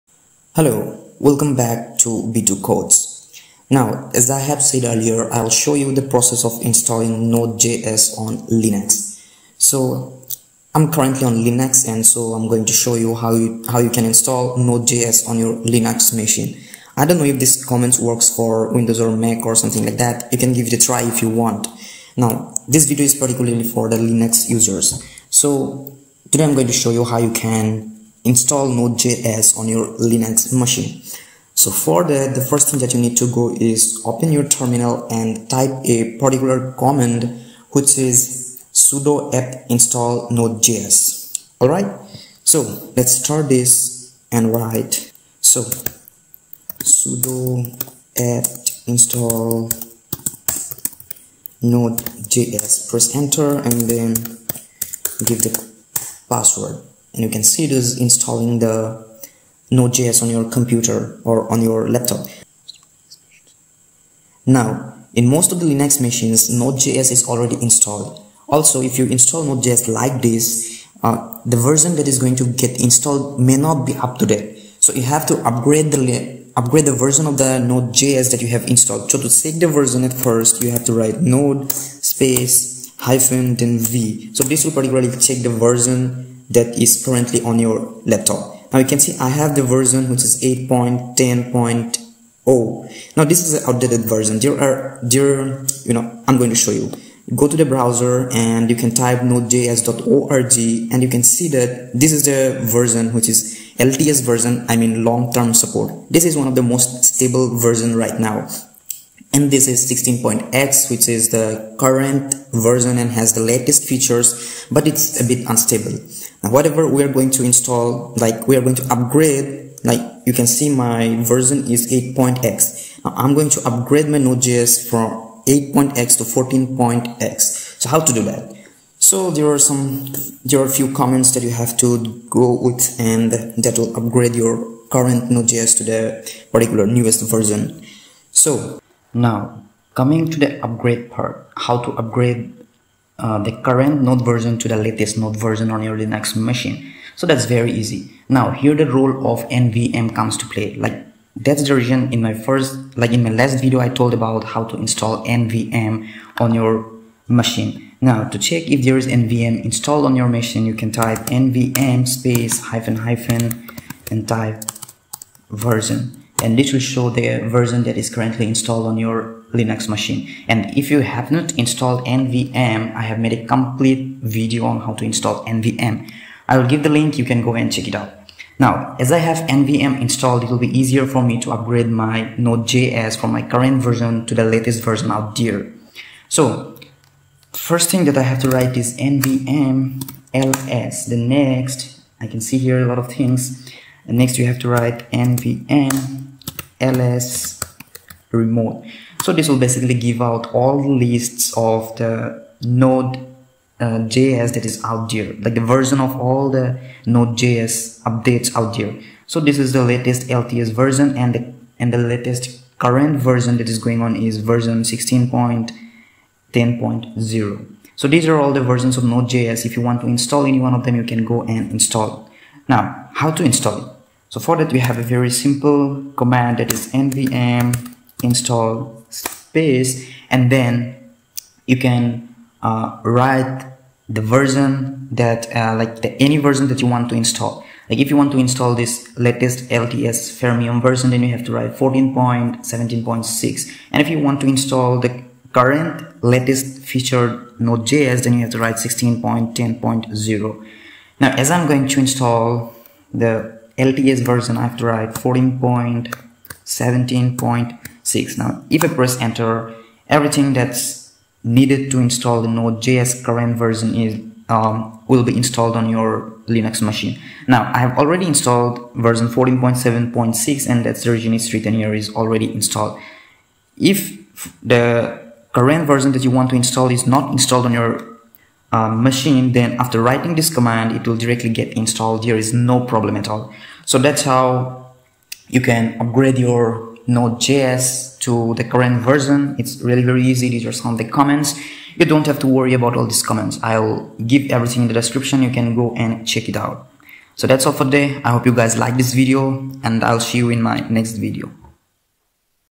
Installing and Upgrading Node js on Linux 🔥
Node.js is an open-source, cross-platform, back-end JavaScript runtime environment that runs on the V8 engine and executes JavaScript code outside a web browser.

In this video I showed how we can install Node js on Linux also how we can upgrade to the latest version using a few linux commands.

You can upgrade to any of the the two versions either LTS or the current one.

Commands -

1. sudo apt install nodejs
2. node -v (verify the version)

Upgrade -

1. nvm --version (check if NVM is installed)

2. nvm ls
3. nvm ls-remote (it'll give all the versions of nodejs available till now)
4. nvm install (version number)

Done ✅